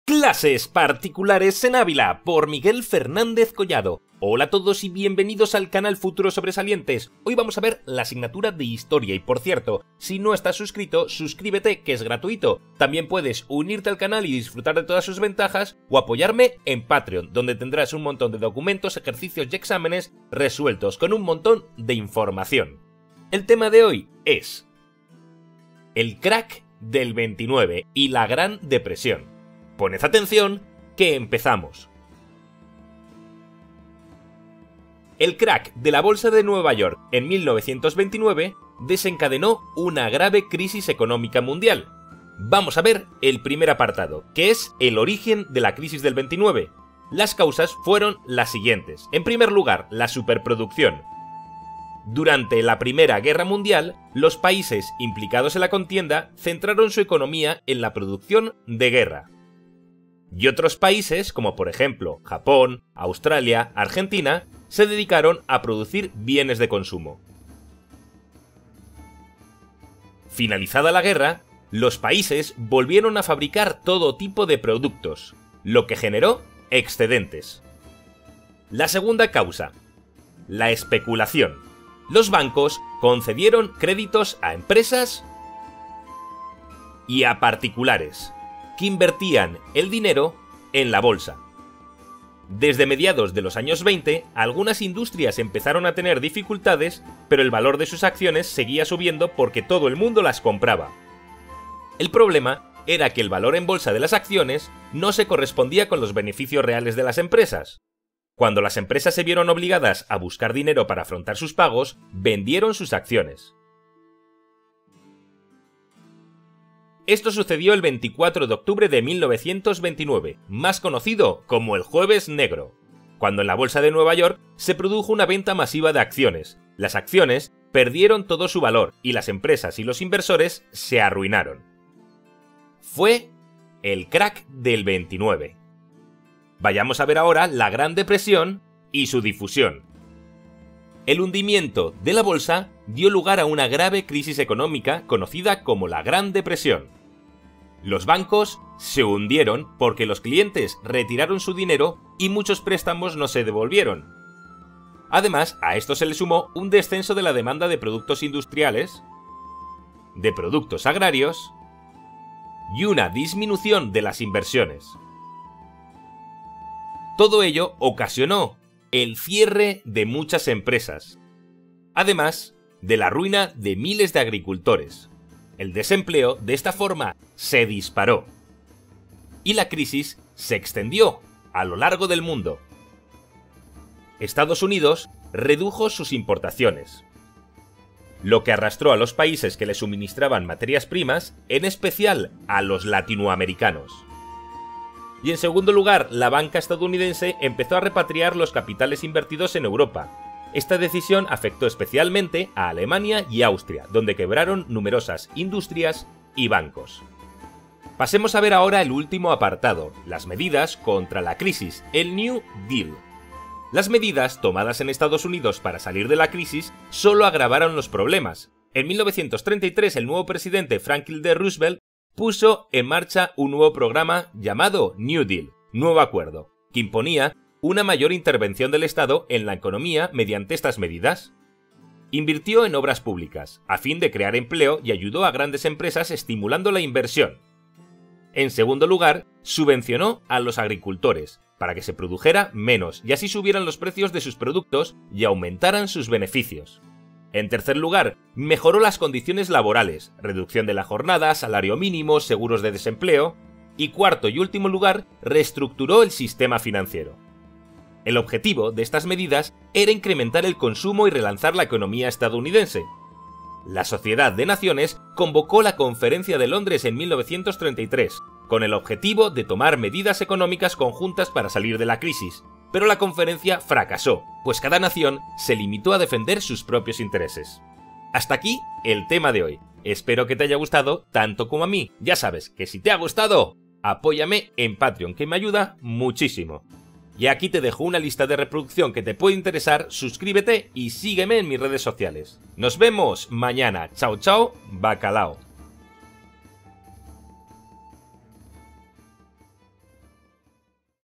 😓 El CRACK del 29 y la GRAN DEPRESIÓN
😓 En este vídeo educativo vamos a ver la Gran Depresión del años 30 explicando un resumen de el crack de la bolsa de Nueva Yorok  de 1929  y la Gran Depresión, viendo:
- El origen de la crisis del 29
- La Gran Depresión y su difusión
- Medidas contra la crisis y el New Deal

⭐ MÁS SOBRE HISTORIA 4º ESO y 1º de BACHILLERATO:⭐

COMPRA en Amazon con este enlace